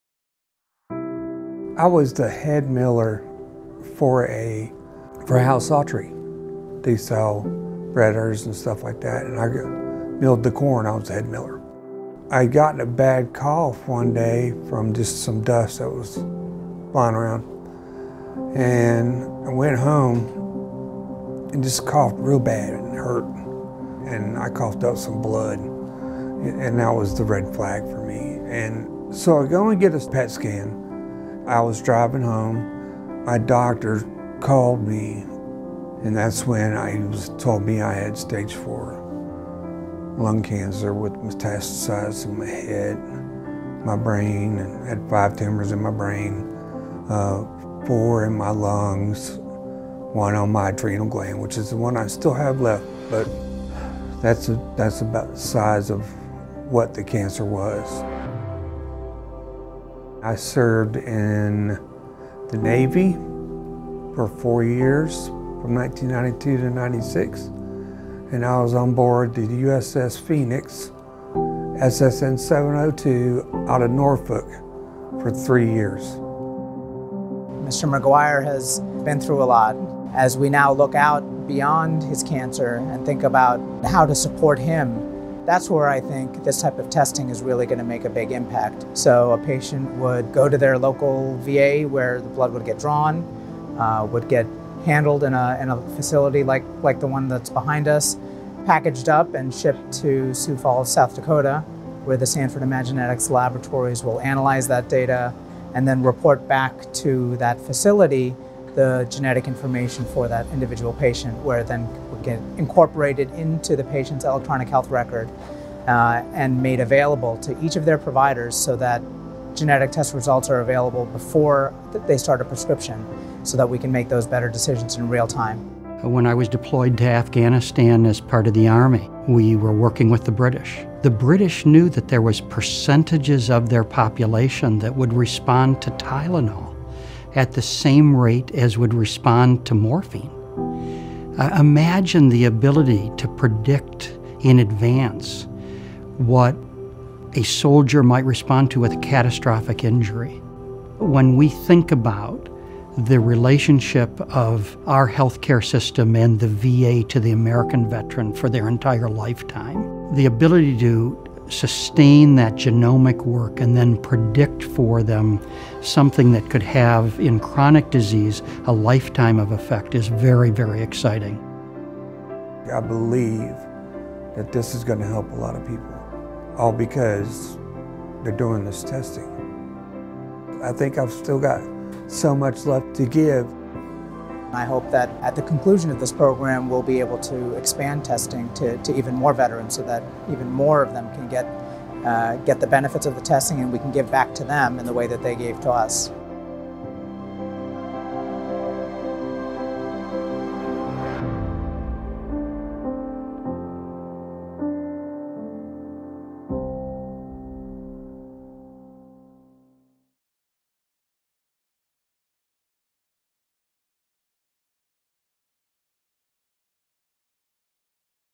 Pat McGuire: I still have so much left to give
A partnership between VA and Sanford Health is helping Veterans by finding the right medications based on their genetic makeup.

Dr. Richard Stone, Veterans Health Administration's Executive in Charge shares a powerful testimonial about his own experience with this testing and the potential this innovative partnership around precision medicine will have.